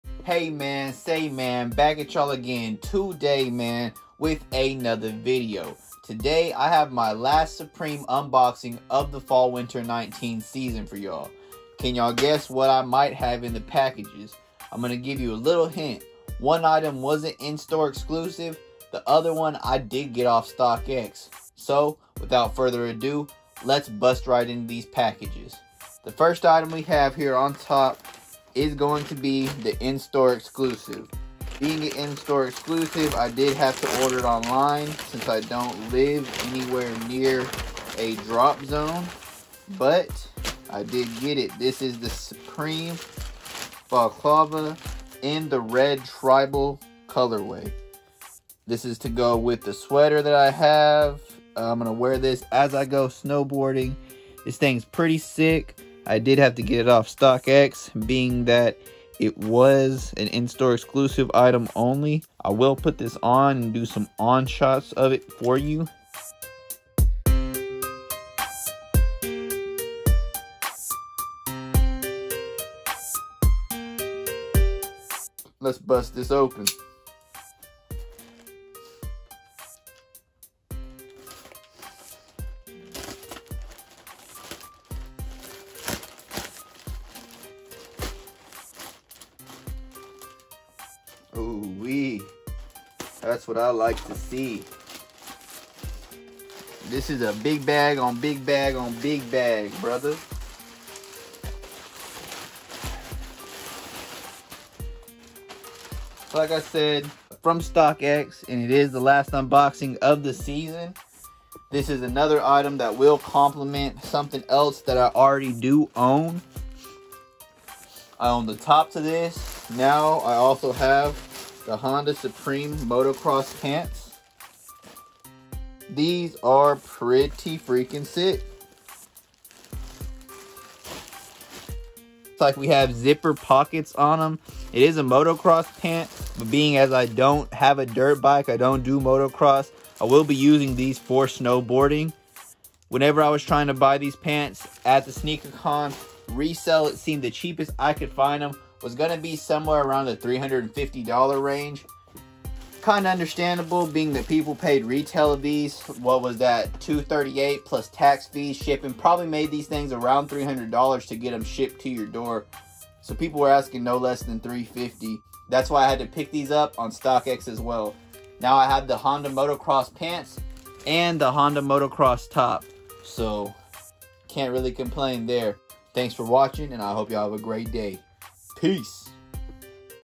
Supreme Red Tribal Balaclava
This is my last supreme unboxing of the fw19 season! Check out the video to see what I got.